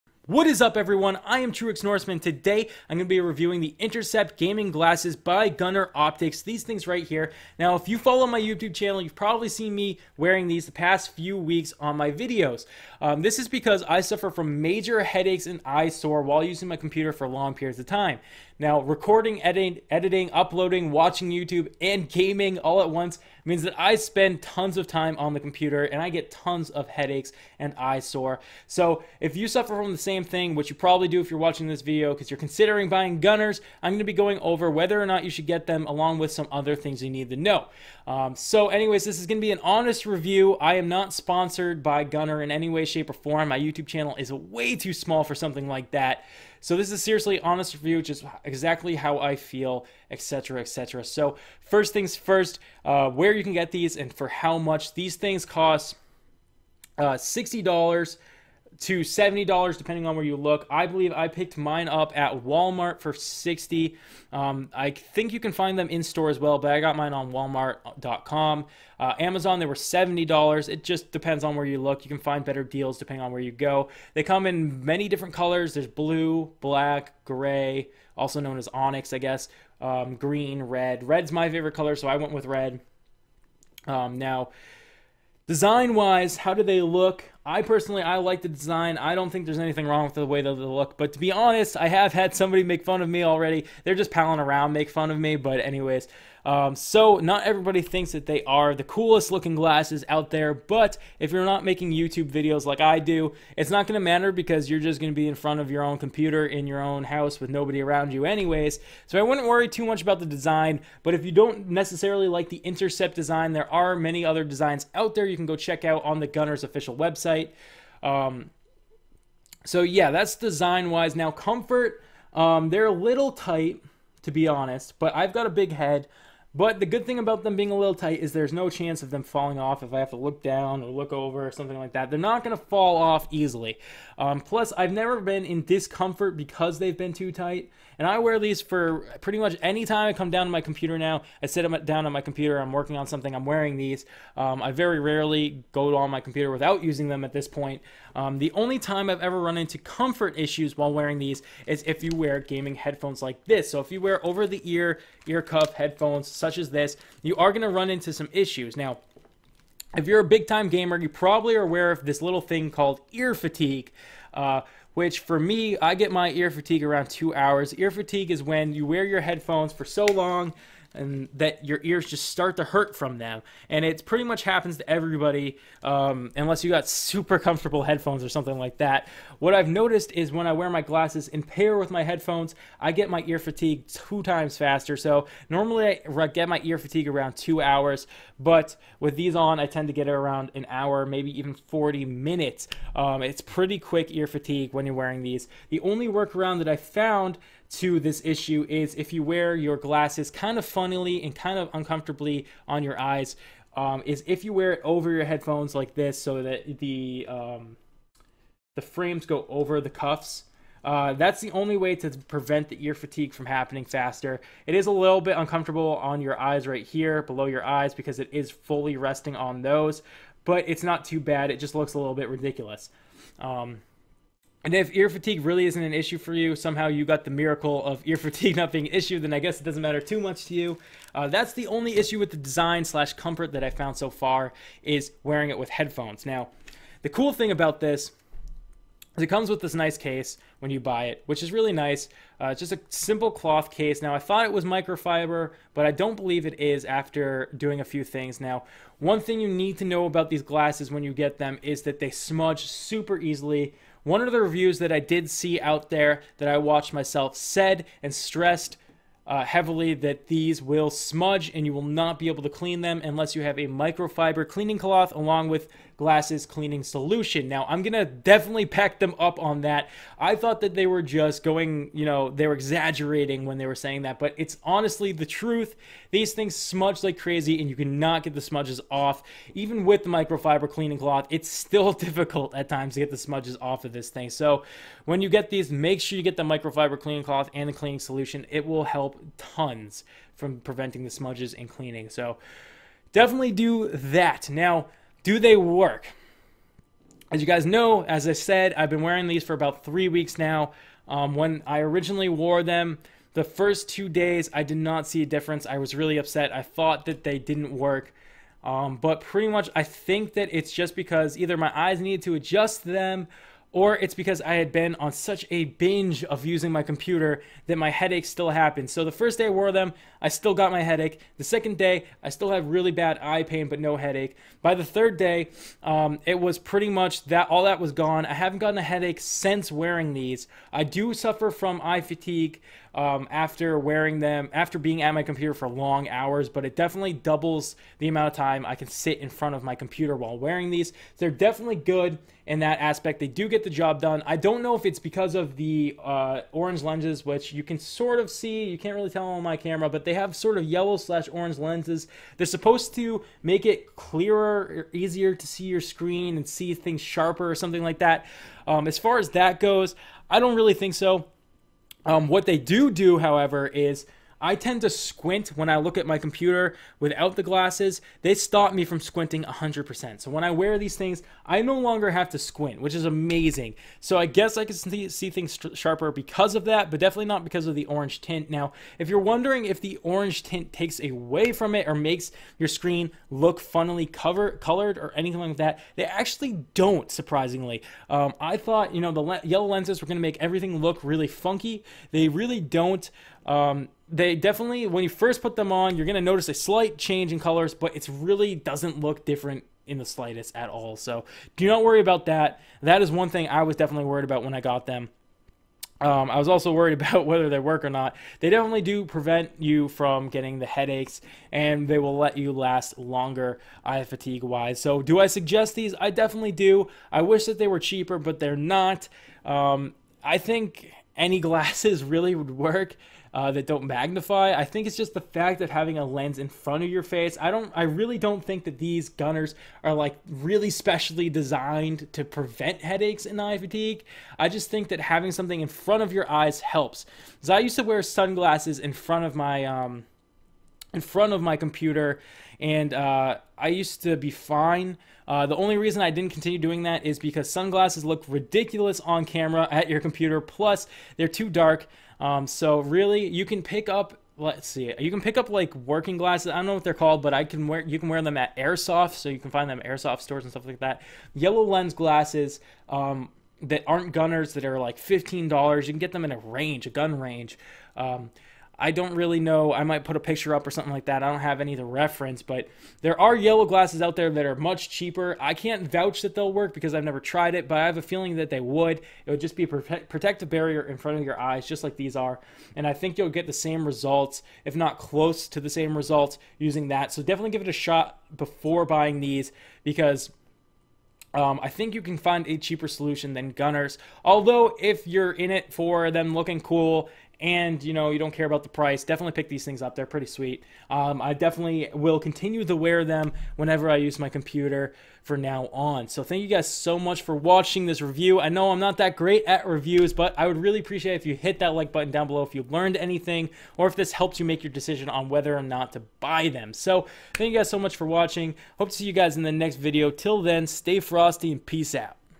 Gunnar Optiks Intercept Gaming Glasses Honest Review
A quick and honest review of the Intercept Gaming Glasses by Gunnar Optiks. I go over design, comfort and performance and hopefully help you with your decision to either buy or not buy this product.